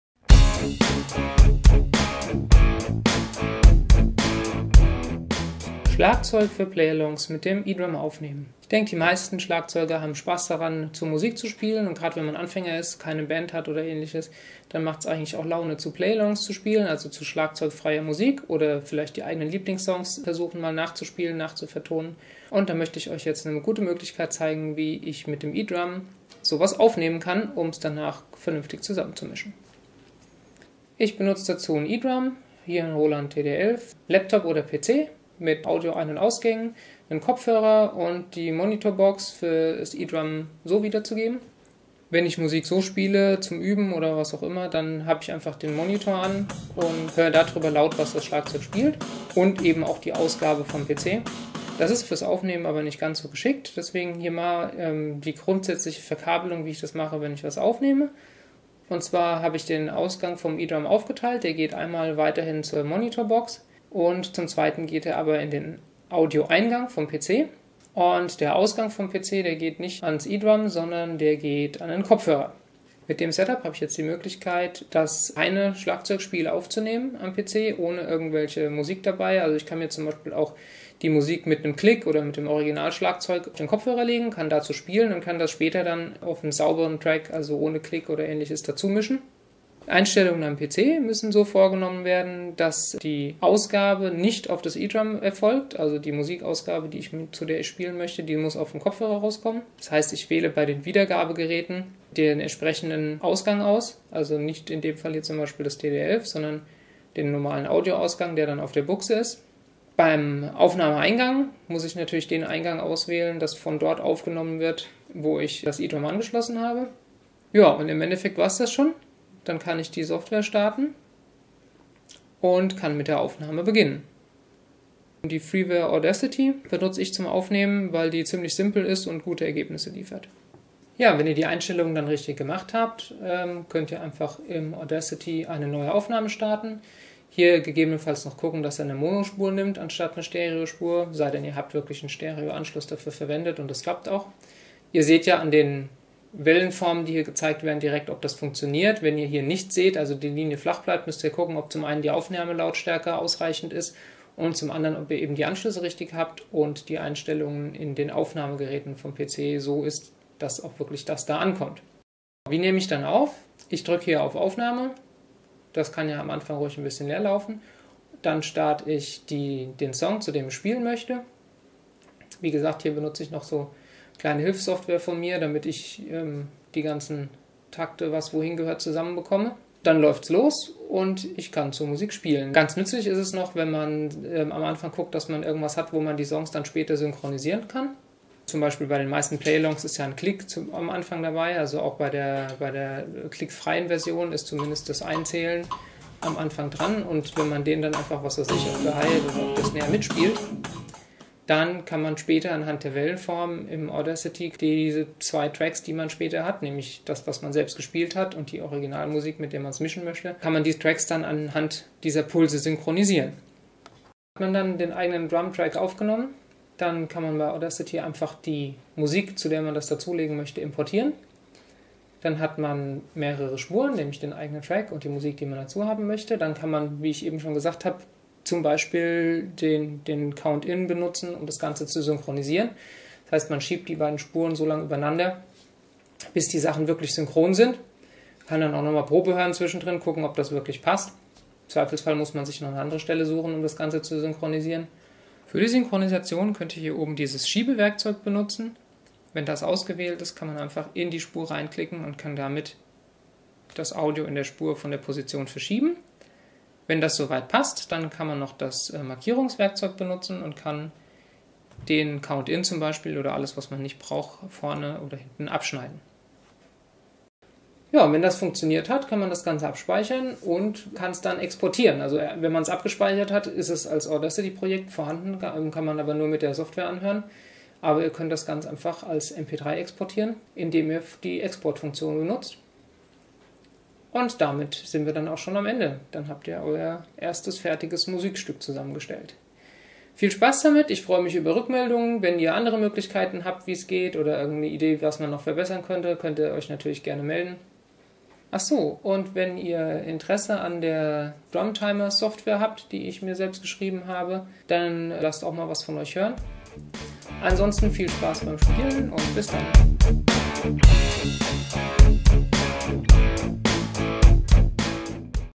E-Schlagzeug aufnehmen für schlagzeuglose Musik oder Play-Alongs
Eine kurze Anleitung wie man mit einem e-Drum Musik für Play-Alongs (etc.) aufnehmen und zusammen mischen kann.
Der Intro- / Outro Song ist z.B. so entstanden.
Wenn Ihr Lob, Fragen oder Anregungen habt, könnte Ihr mir gerne einen Kommentar schreiben.